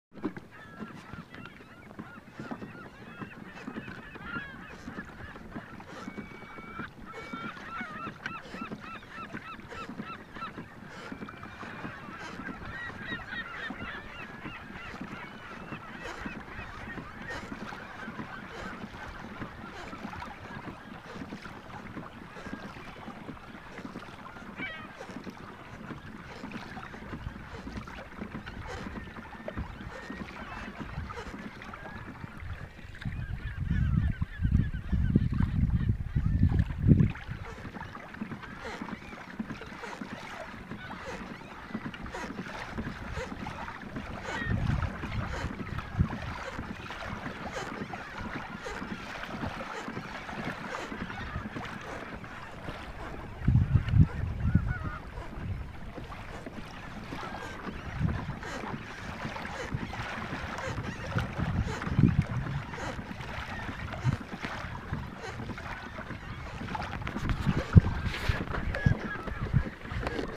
Birds Of Lake Perris
My travels across Lake Perris during a kayak fishing trip on 3/16/2016 took me through a flock of birds.